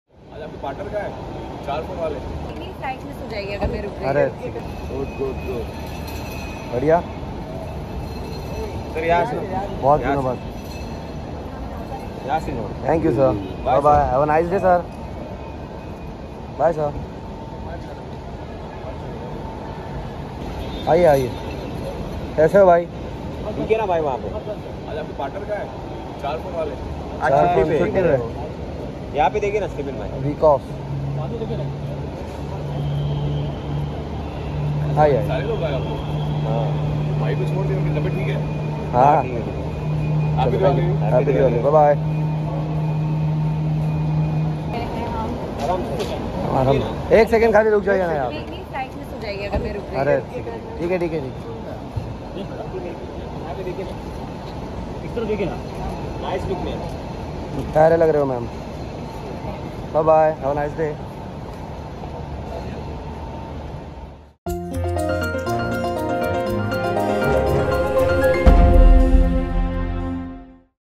Sanjay Mishra, Stebin Ben & Nupur Sanon Gets Snapped At The Mumbai Aiport In Full Swag!
B-Town Talents: Sanjay Mishra Stebin Ben & Nupur Sanon get clicked at the Mumbai Airport in their classy avatars!